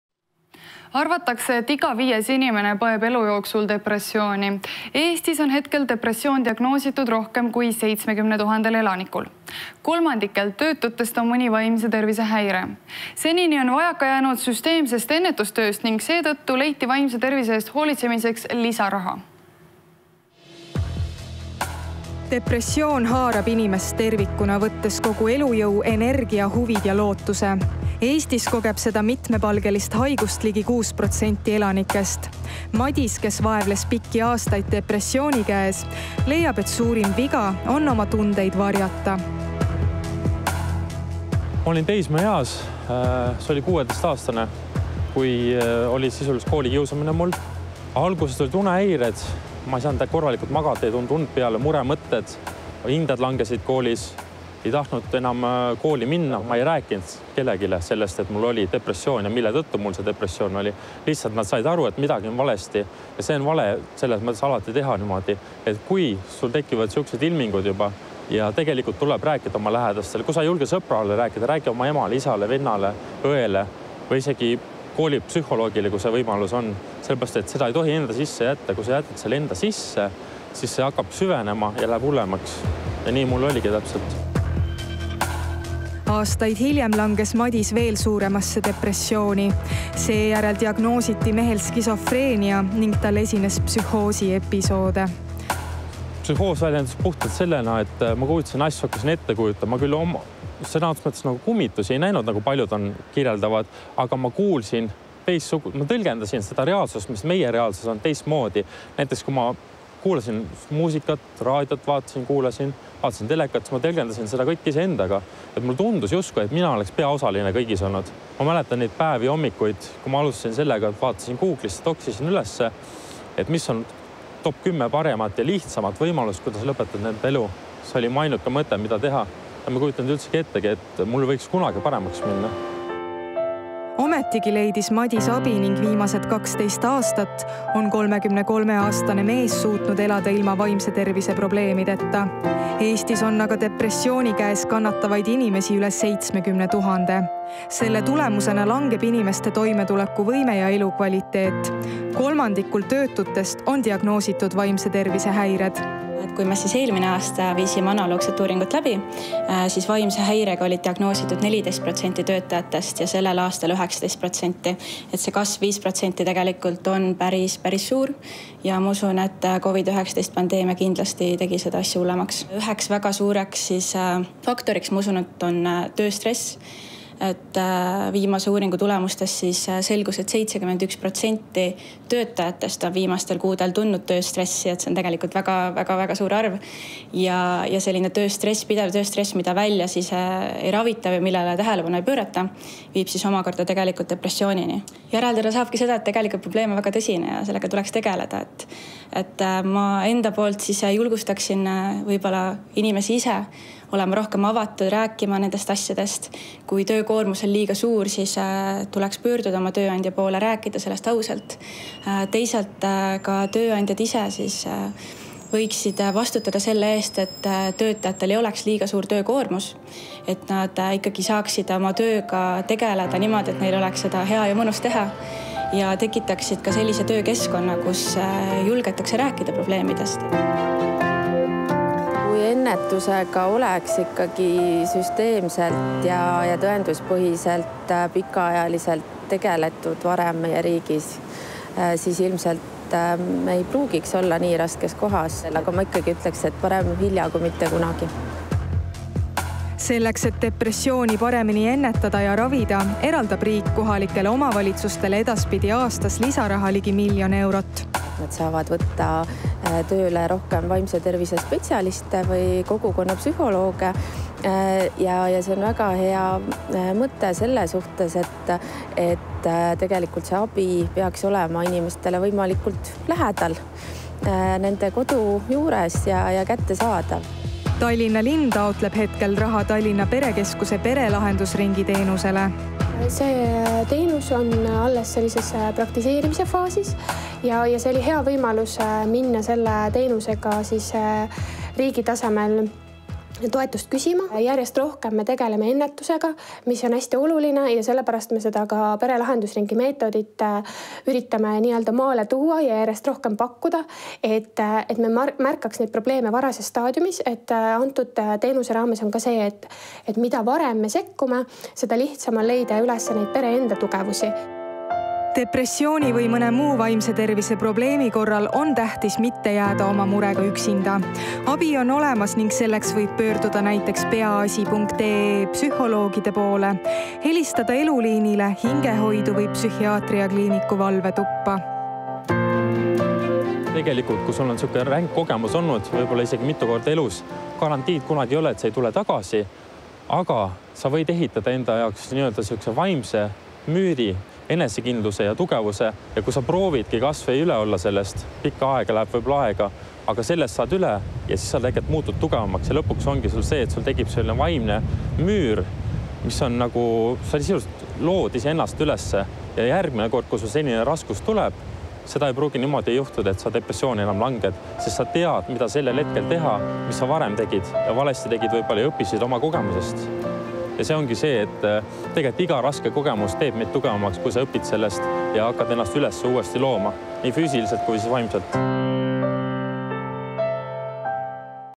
24.10.2021 - Lisarahastus vaimse tervise häirete jaoks
Arvatakse, et iga viies inimene põeb elu jooksul depressiooni.
Eestis on hetkel depressioon diagnoositud rohkem kui 70 tuhandel
elanikul. Kolmandikul töötutest on mõni vaimse tervise häire.
Senini on  vajaka jäänud süsteemsest ennetustööst ning seetõttu leiti
vaimse tervise eest hoolitsemiseks lisaraha.